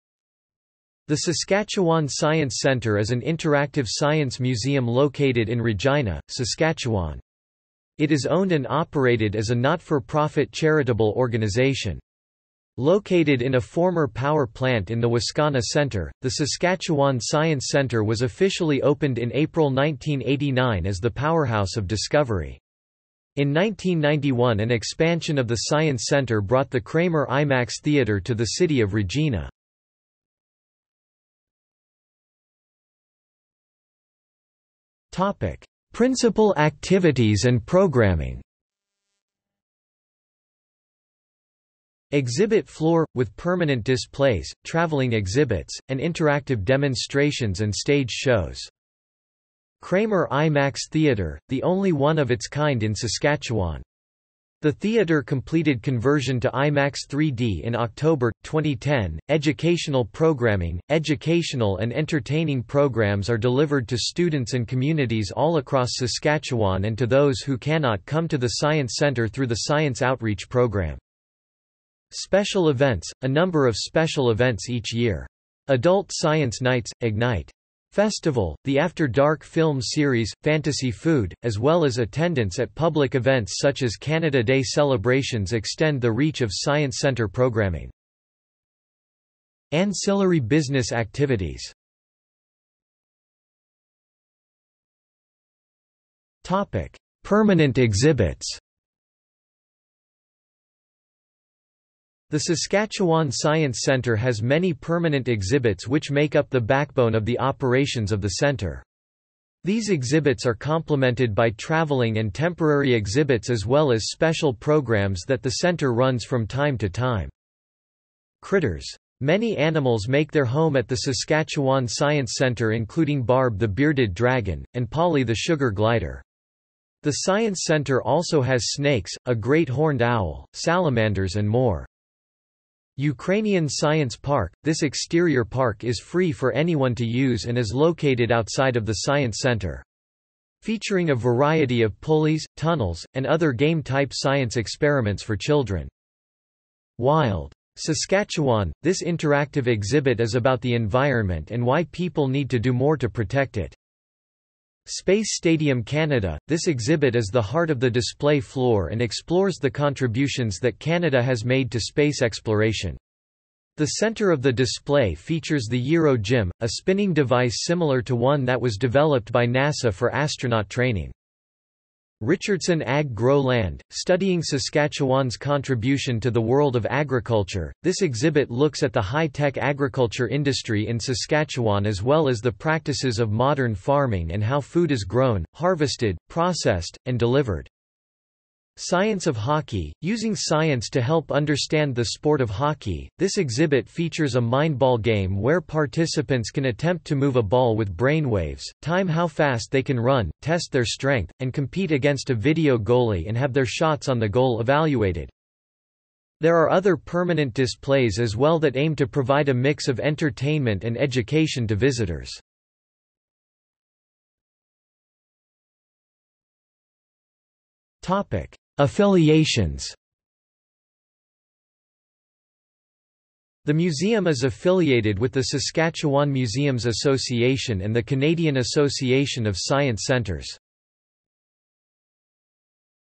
Saskatchewan Science Centre | Wikipedia audio article
00:00:31 1 Principal Activities and Programming
00:01:32 2 Permanent Exhibits
00:03:34 3 Affiliations

- Socrates

SUMMARY
The Saskatchewan Science Centre is an interactive science museum located in Regina, Saskatchewan. It is owned and operated as a not-for-profit charitable organization. Located in a former power plant in the Wascana Centre, the Saskatchewan Science Centre was officially opened in April 1989 as the Powerhouse of Discovery. In 1991 an expansion of the Science Centre brought the Kramer IMAX Theatre to the City of Regina.